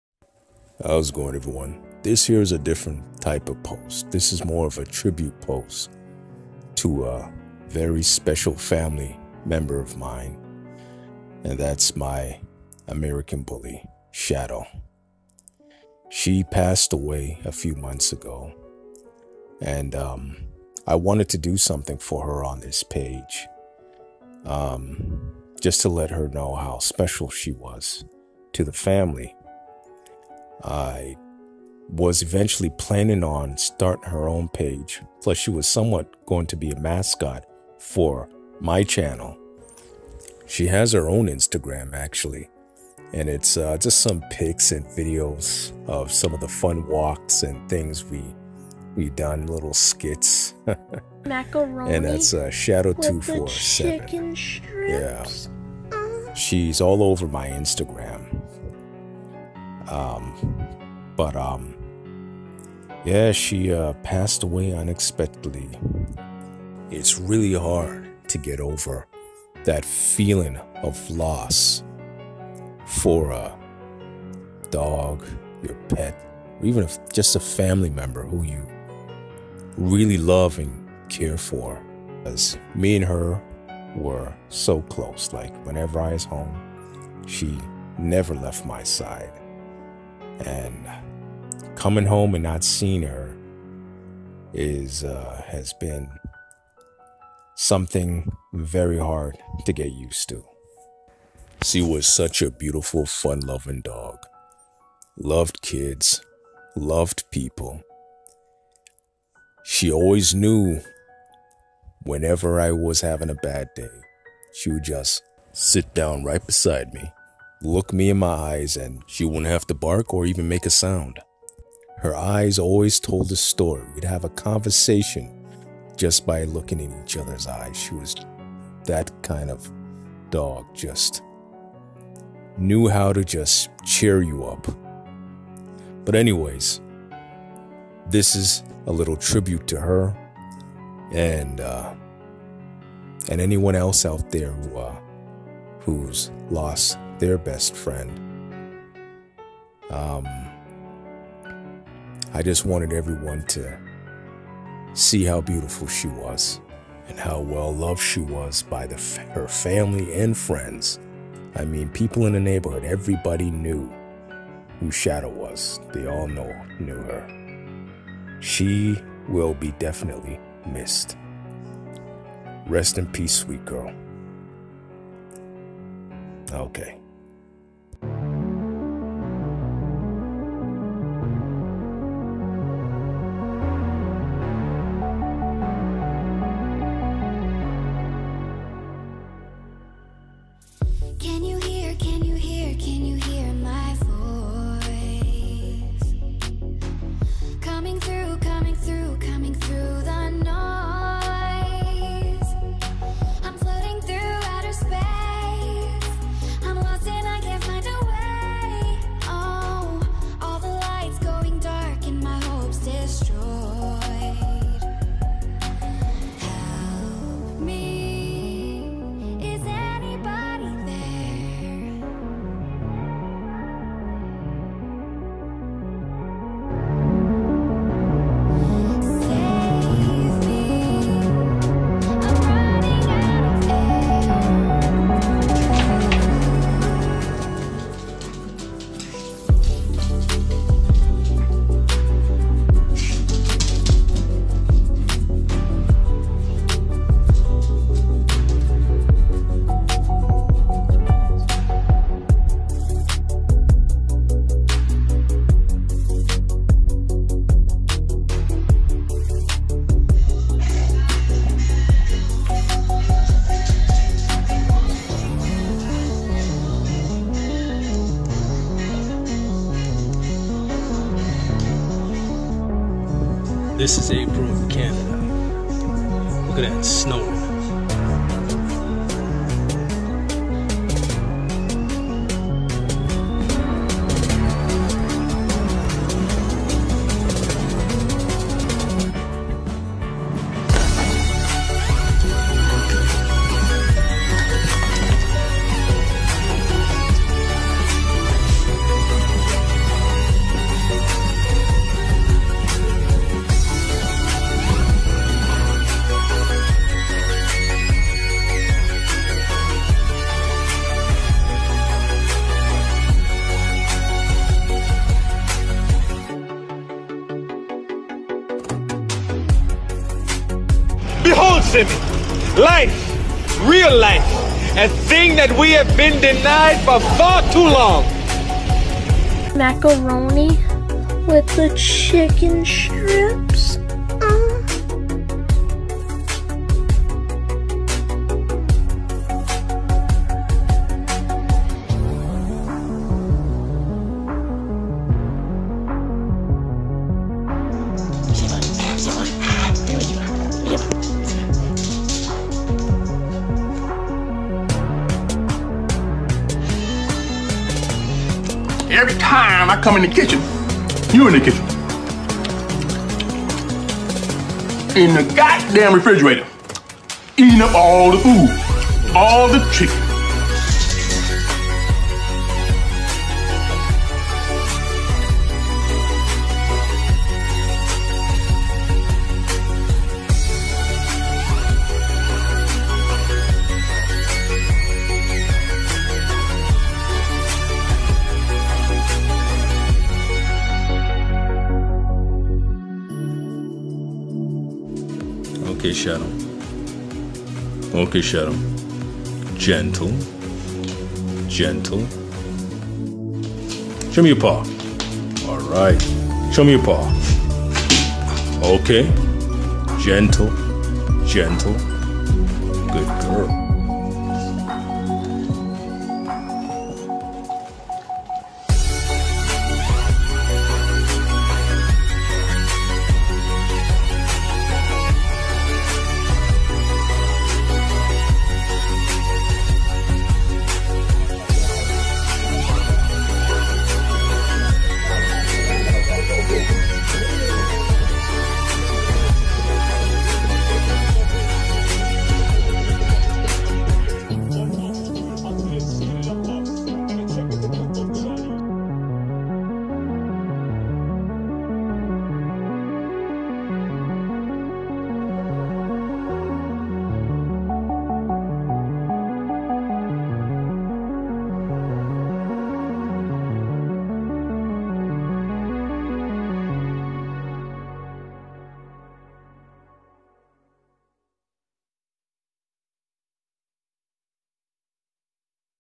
Tribute To My American bully Shadow RIP
Dogs' lives are short, too short, but you know that going in. You know the pain is coming, you're going to lose a friend, and there's going to be great anguish, so you live fully in the moment with them, never fail to share their joy or delight in their innocence, because you can't support the illusion that a dog can be your lifelong companion. There's such beauty in the hard honesty of that, in accepting and giving love while always aware that it comes with an unbearable price."
RIP Shadow